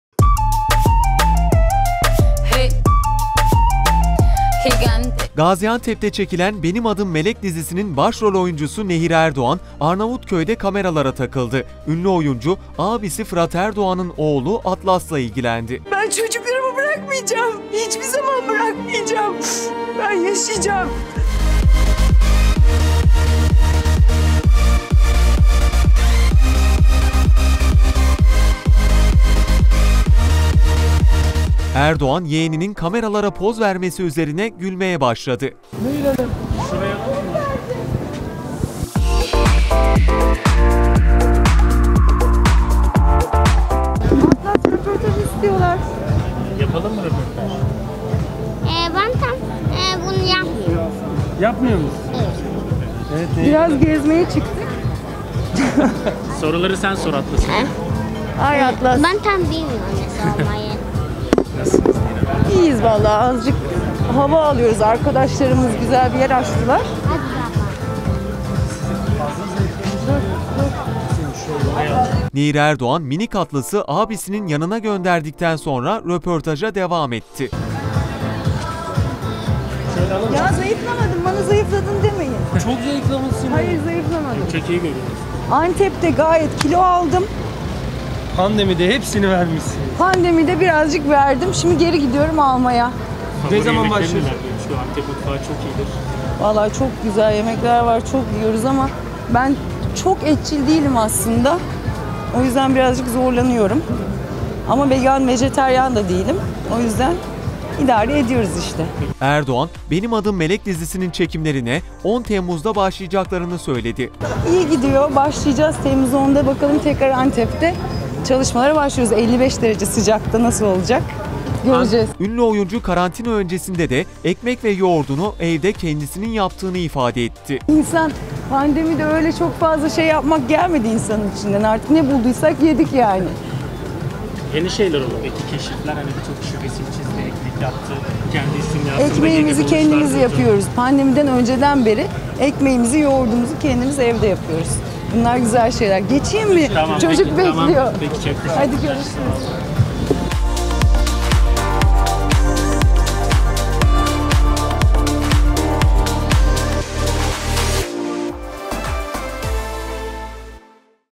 Nehir Erdoğan'ın zayıflık isyanı
Gaziantep'te çekilen "Benim Adım Melek" dizisinin başrol oyuncusu Nehir Erdoğna, Arnavutköy'de kameralara takıldı. Ünlü oyuncu abisi Fırat Erdoğan'ın oğlu Atlas'la ilgilendi.

Erdoğan yeğeninin kameralara poz vermesi üzerine gülmeye başladı.

"Benim Adım Melek" dizisinin çekimlerine 10 Temmuz'da başlayacaklarını anlatan Erdoğan, karantina öncesinde ekmek ve yoğurduğunu evde kendisinin yaptığını söyledi.

tv100 uydu bilgileri:

TÜRKSAT 4A
Frekans: 12729
Sembol: 30000
Polarizasyon: V (Dikey)